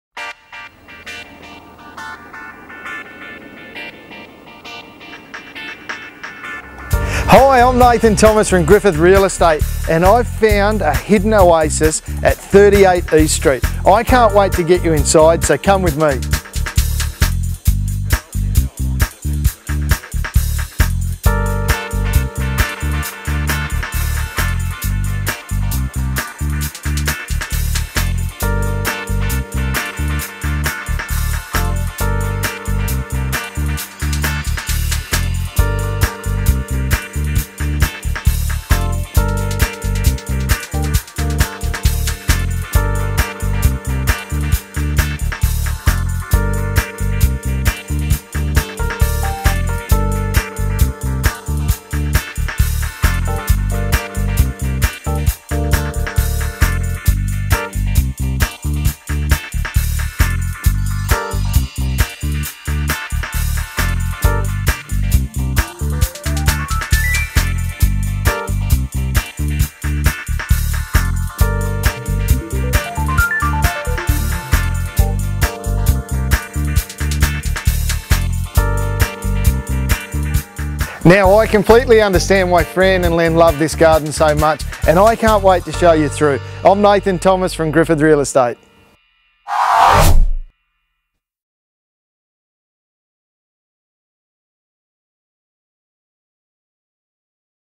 38 East Street, Yoogali
Family retreat . . .
Open the door to a new way of life -- style, comfort and convenience.
This spacious designer home boasts two living areas, a gourmet kitchen and sun-drenched northerly aspect.
Entertain year-round -- the living spaces flow freely, inside to out.
The property includes a double garage, drive-through access to the big backyard and a garden shed.
It's everything you could want. Call Nathan Thomas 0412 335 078